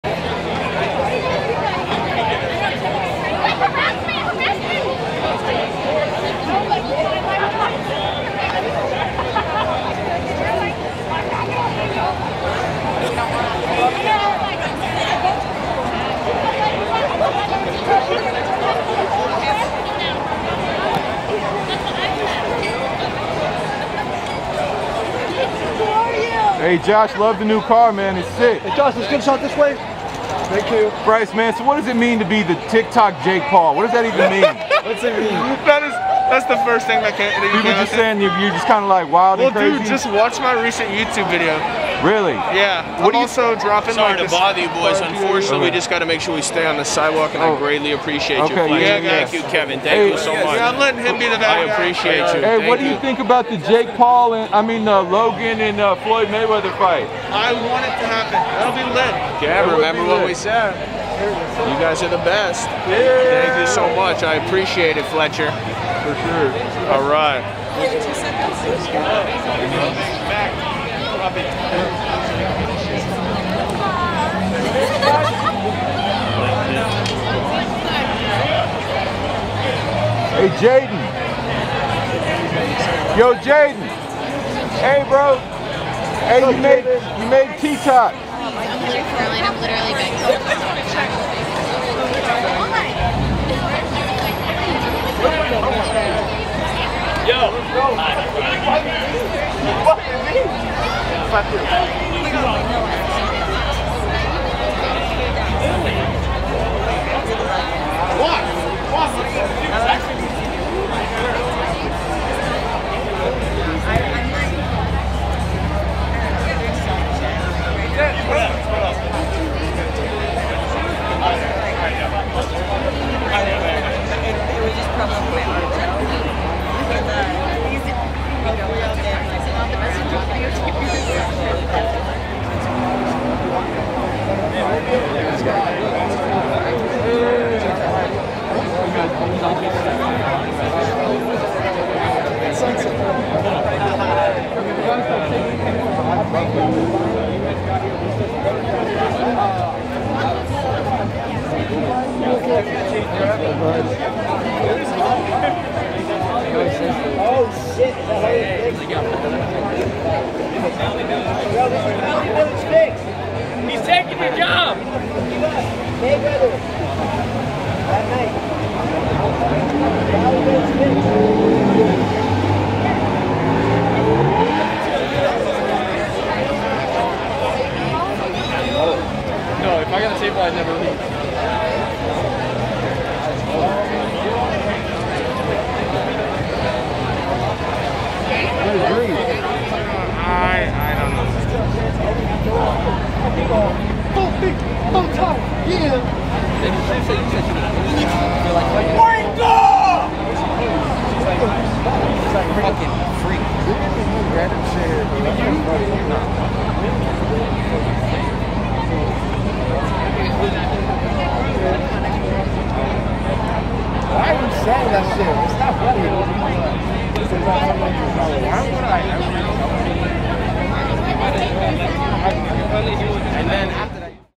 Bryce Hall Speaks On Being The TikTok Version Of Jake Paul While Taking Shots With Josh Richards
BryceHall Speaks On Being The TikTok Version Of JakePaul While Taking Shots With JoshRichards 9.17.20 - TheHollywoodFix